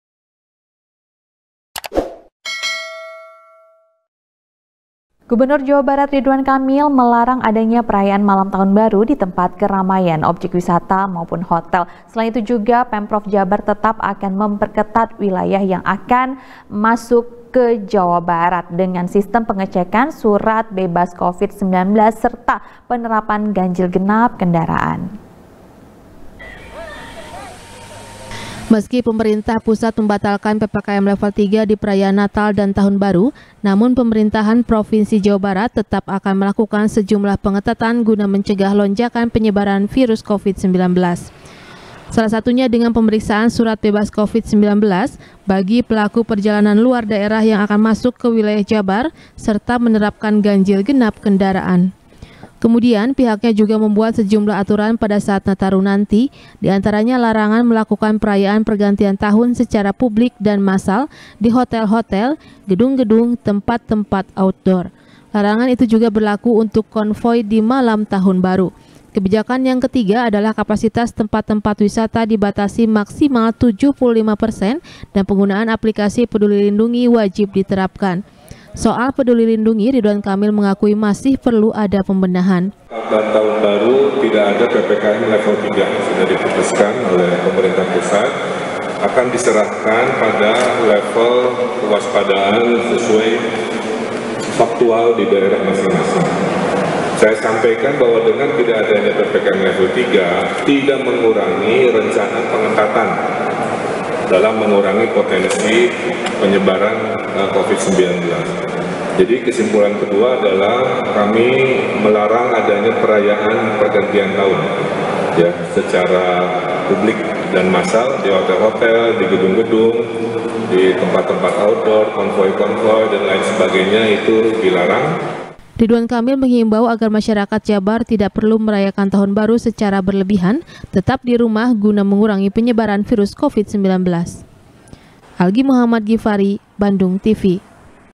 Pengetatan Nataru Dan Larangan Perayaan Tahun Baru | SBR | BANDUNGTV
SBR | BANDUNGTV | KRISANTI | Gubernur Jawa Barat Ridwan Kamil Melarang Adanya Perayaan Malam Tahun Baru Di Tempat Keramaian,.Objek Wisata Maupun Di Hotel.Selain Itu Juga Pemprov Jabar Tetap Akan Memperketat Wilayah Yang Akan Masuk Ke Jabar,. Dengan Sistem Pengecekan Surat Bebas Covid 19,. Serta Penerapan Ganjil Genap Kendaraan.